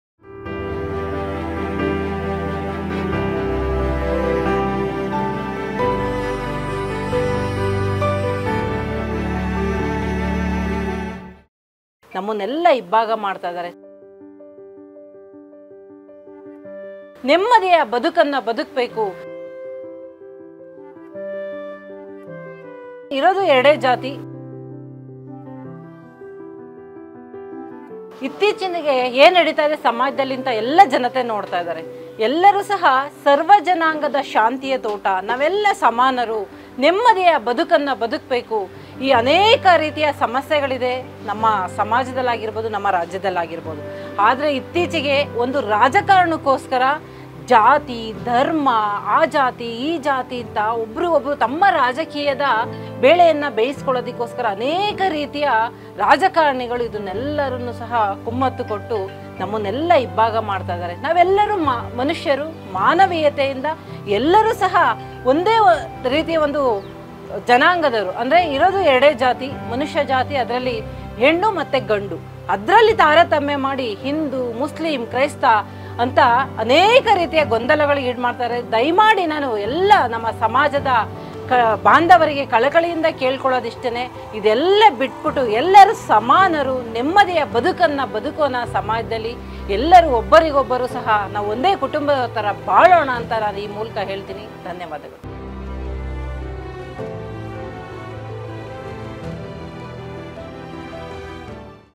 ರಾಜಕಾರಣಿಗಳು ತಮ್ಮ ರಾಜಕೀಯದ ಬೇಳೆಯನ್ನು ಬೇಯ್ಯಿಸಿಕೊಳ್ಳೋದಕ್ಕೆ ಜಾತಿಯನ್ನು ಬಳಸಿಕೊಳ್ತಾ ಇದ್ದಾರೆ...| Secular Tv
ಸರ್ವ ಜನಾಂಗದ ಶಾಂತಿಯ ತೋಟ : ರಾಜಕಾರಣಿಗಳು ತಮ್ಮ ರಾಜಕೀಯದ ಬೇಳೆಯನ್ನು ಬೇಯ್ಯಿಸಿಕೊಳ್ಳೋದಕ್ಕೆ ಜಾತಿಯನ್ನು ಬಳಸಿಕೊಳ್ತಾ ಇದ್ದಾರೆ...| Secular Tv